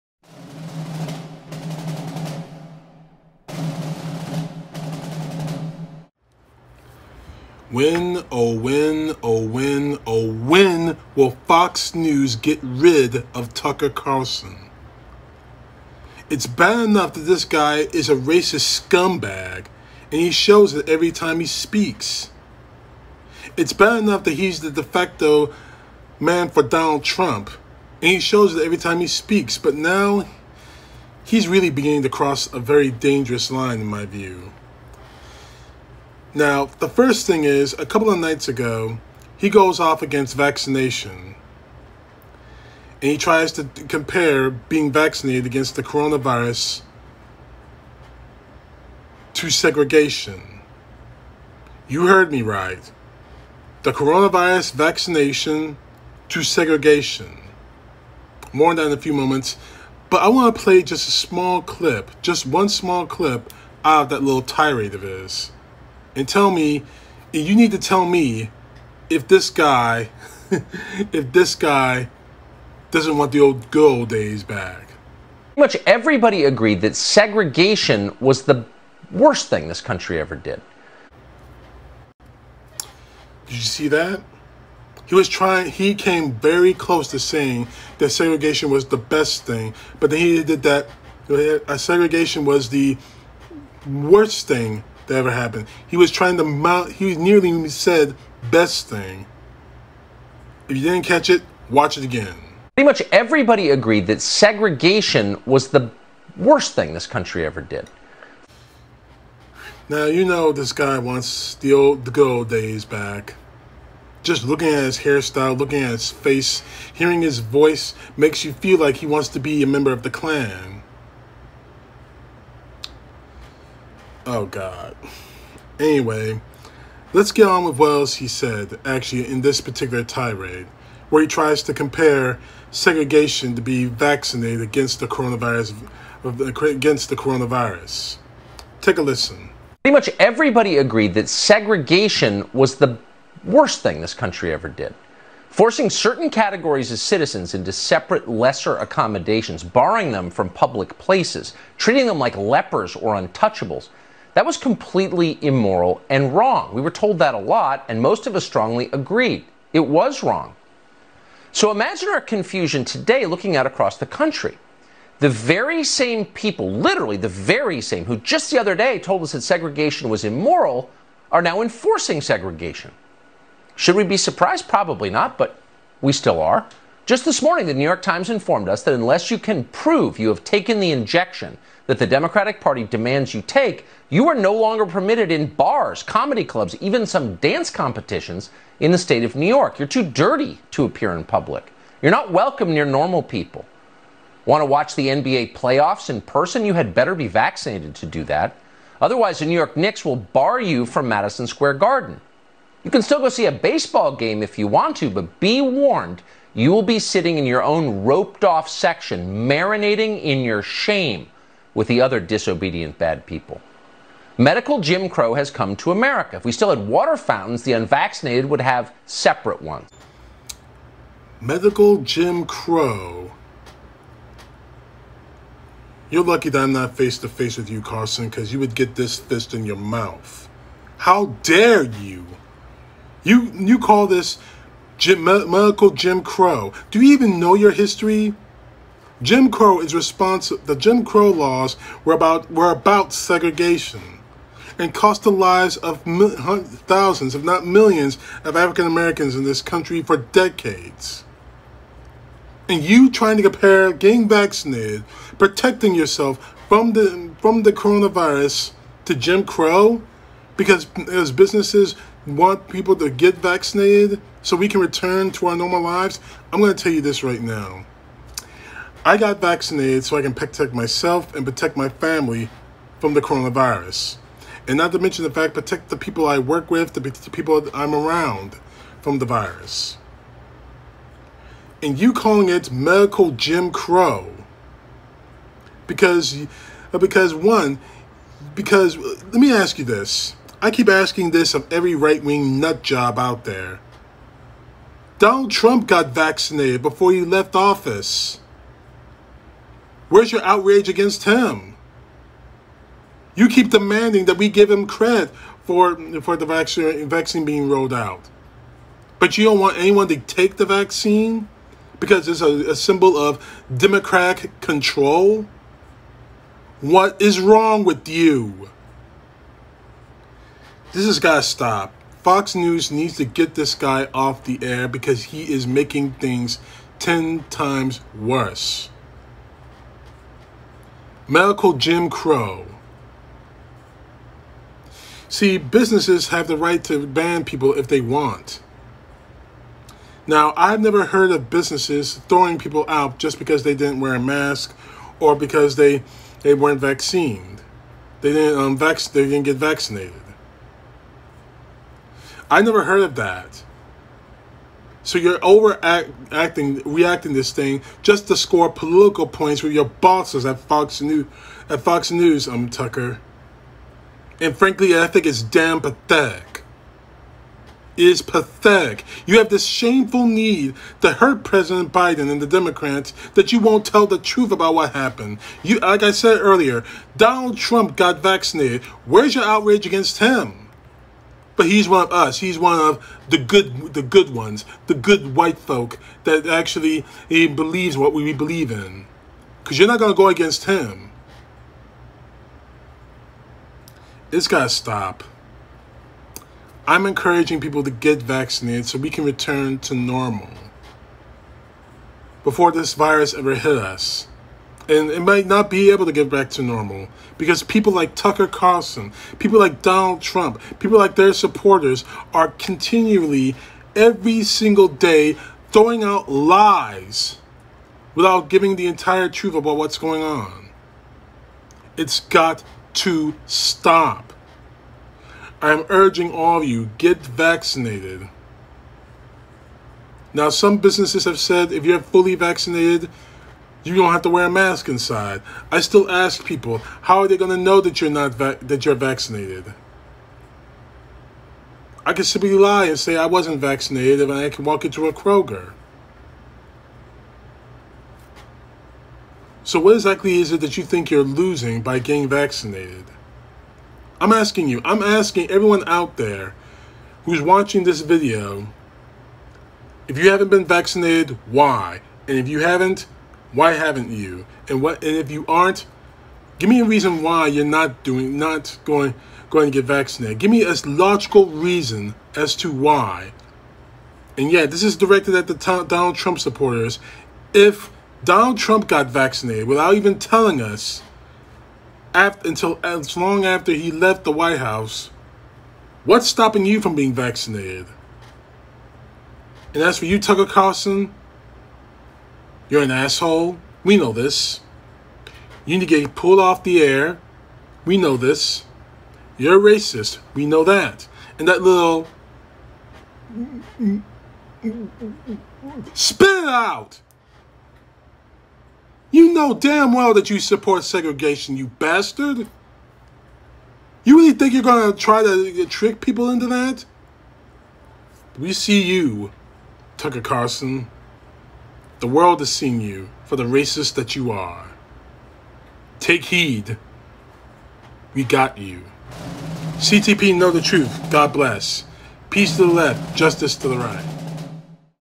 CTP Know The Truth June 4 2021 Medical “Jim Crow” according to Tucker Carlson
Fox News is Tucker Carlson has now compared getting vaccinated against the coronavirus to Jim Crow segregation